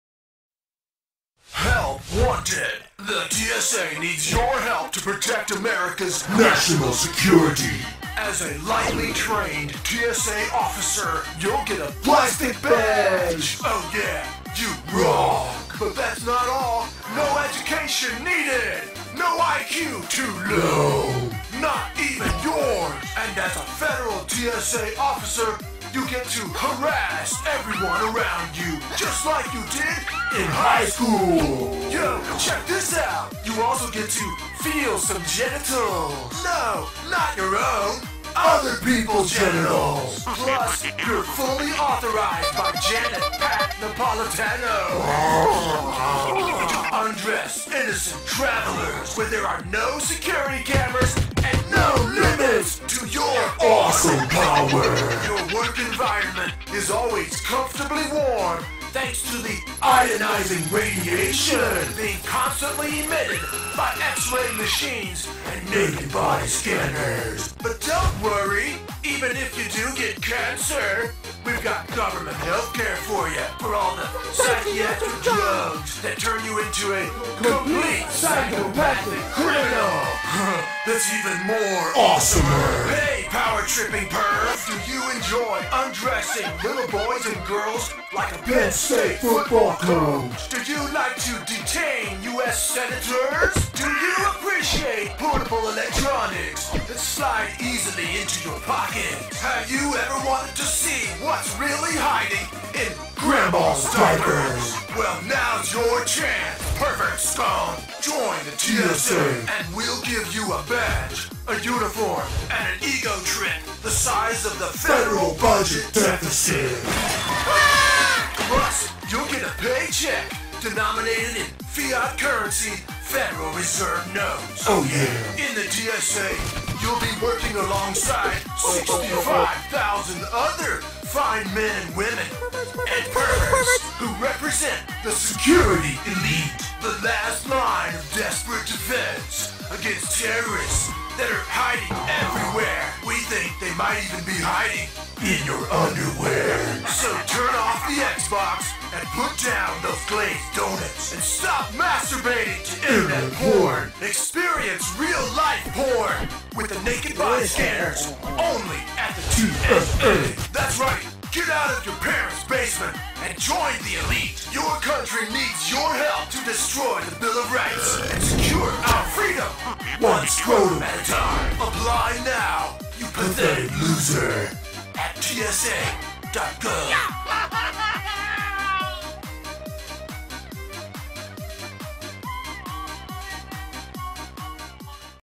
TSA_ Bullies, Thieves, Rapist and Child Molesters
The latest activism video from Mike Adams, the Health Ranger, hilariously depicts "TSA perverts" watching naked body scanner porn, stealing expensive electronics from the luggage of air travelers, digging for crotch gold during "enhanced pat downs" and turning into total psychopathic criminals.